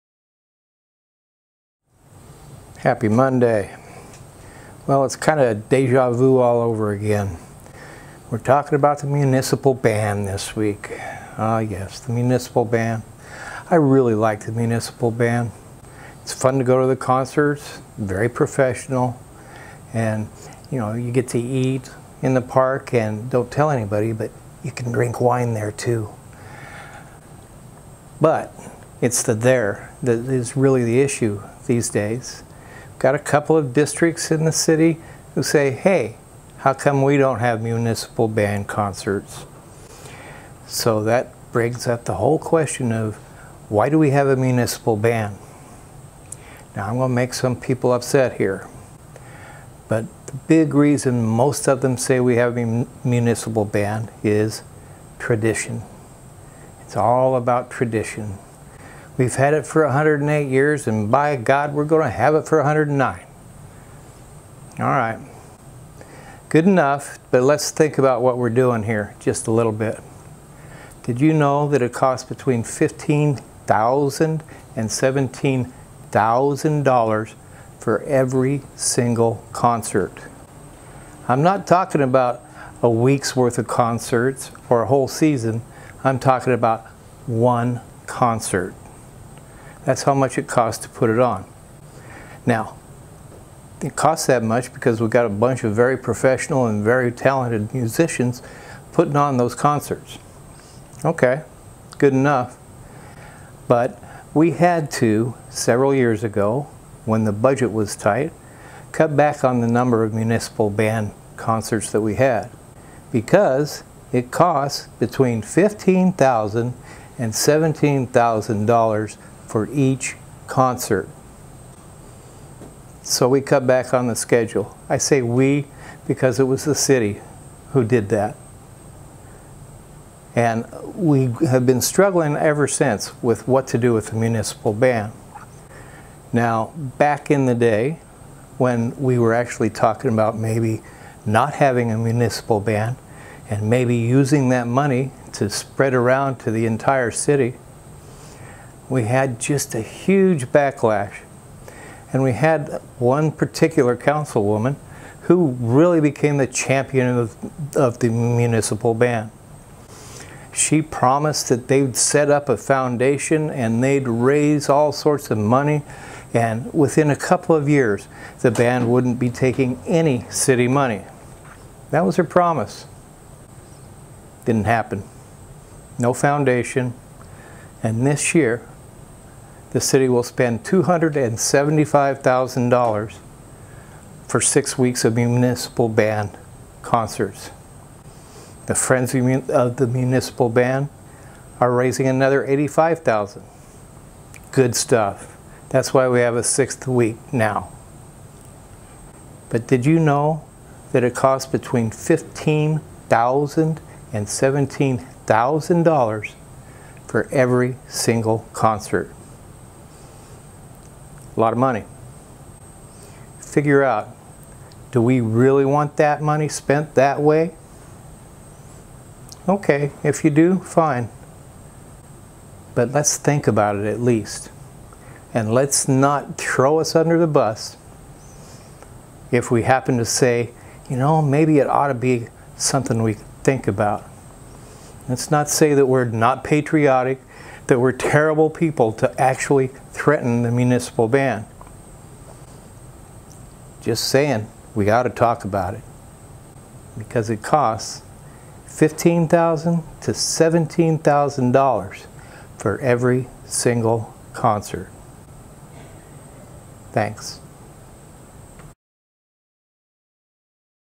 The Rant for week of July 10, 2017
Is the Municipal Band Really Worth the Money Being Spent on It?